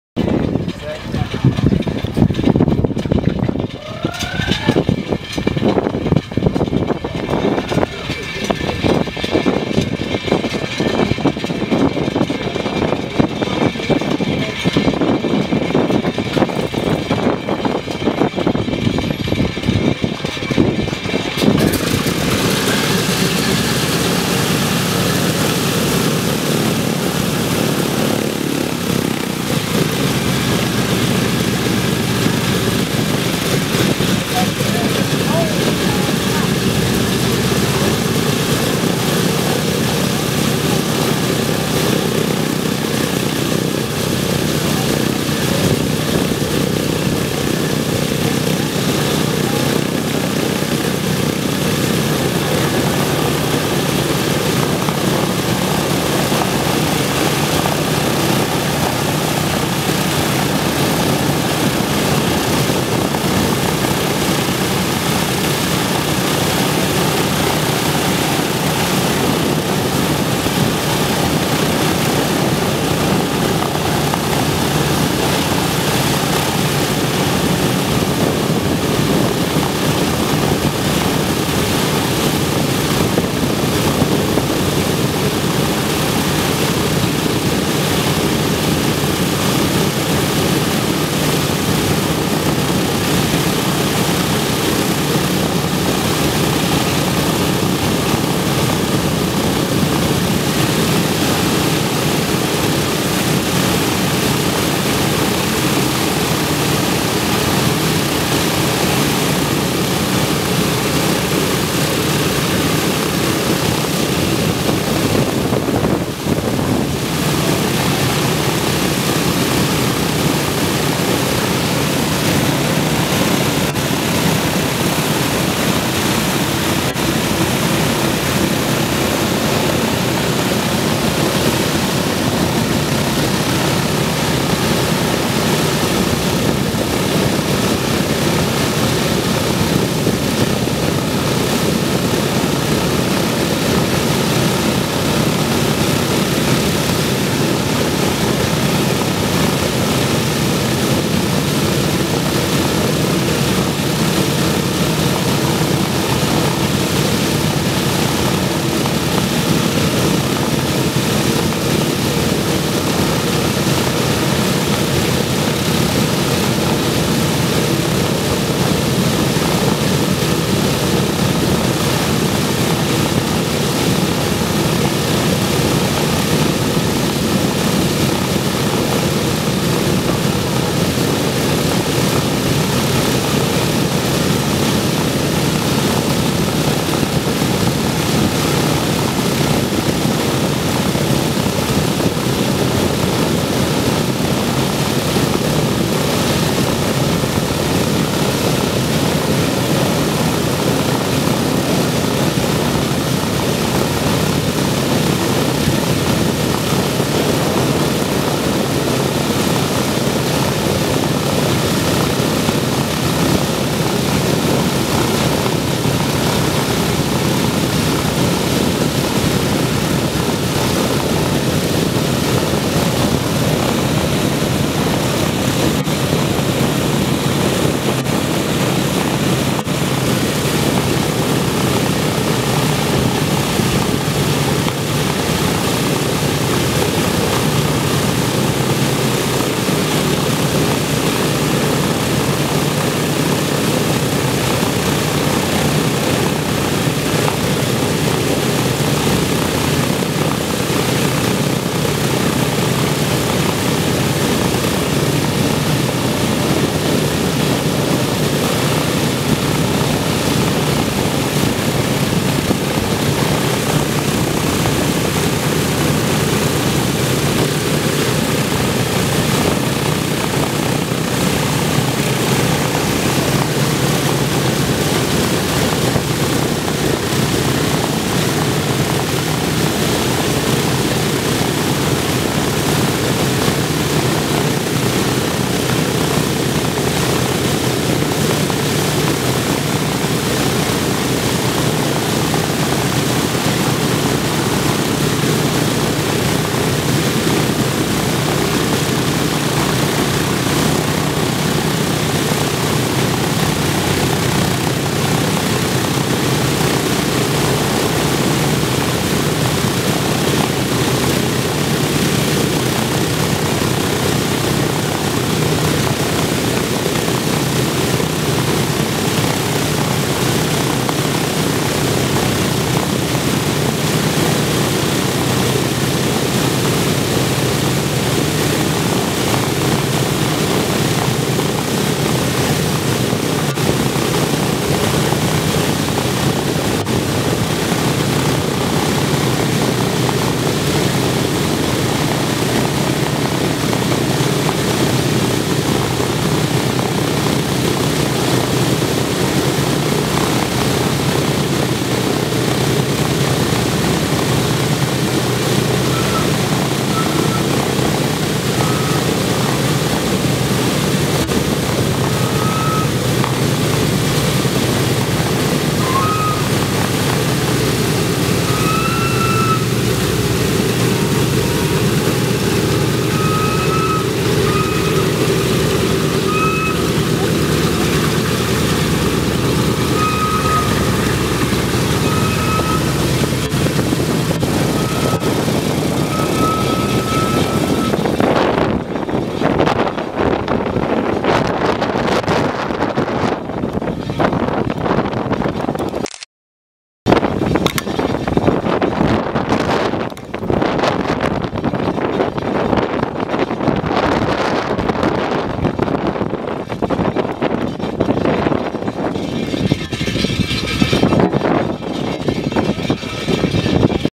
B17 engine startup
Start up and taxi for takeoff